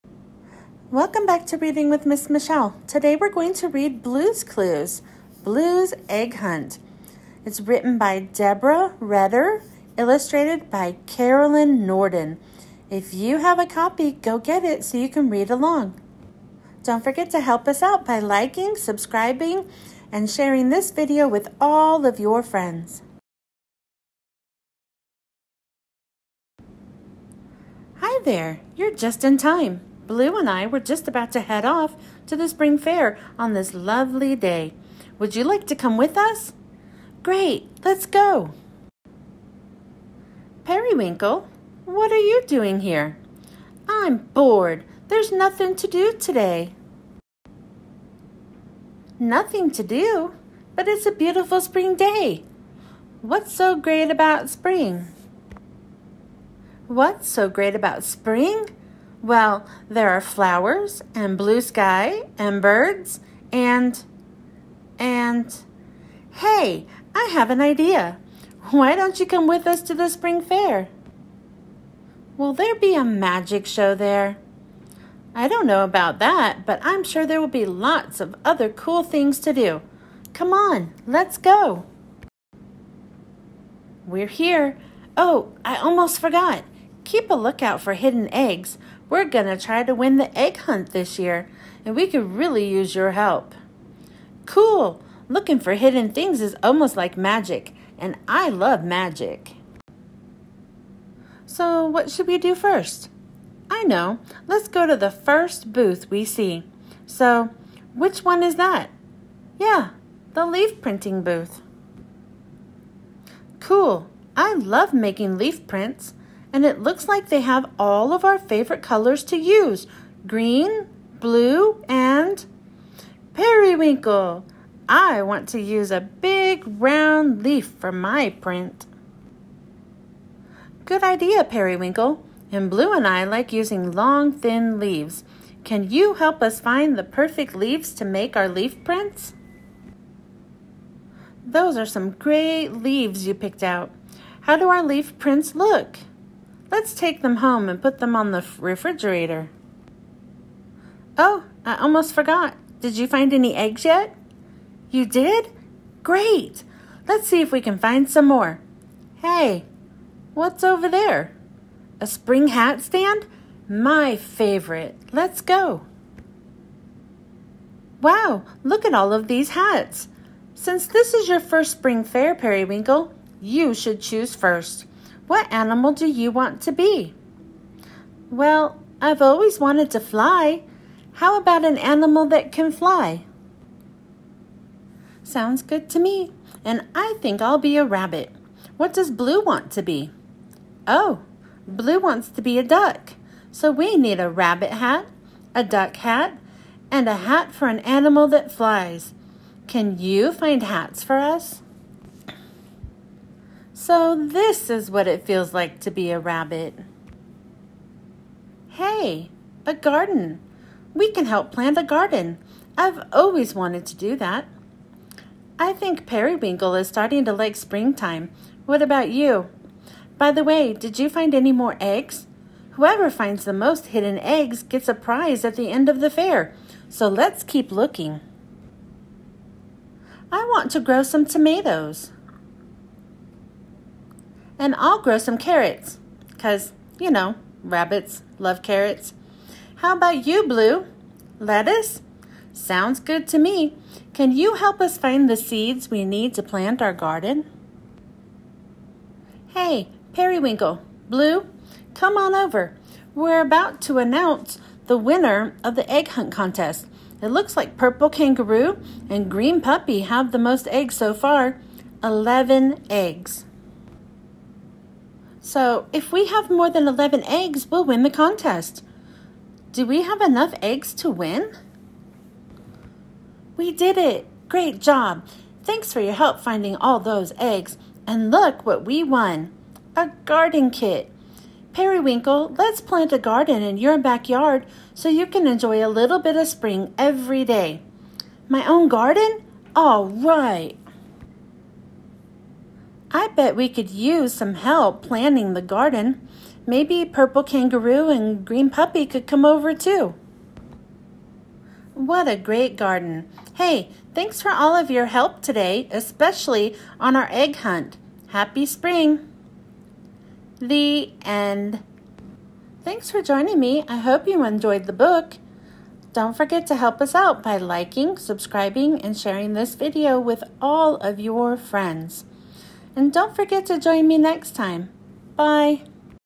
Kids Book Read Aloud: Blue's Clues Blue's Egg Hunt by Deborah Reder and Carolyn Norden Easter
Today we are reading
Blue's Clues Blue's Egg Hunt
Written By Deborah Reder
Illustrated by Carolyn Norden

Thank you for joining Ms. Michelle as we read lots of fun books.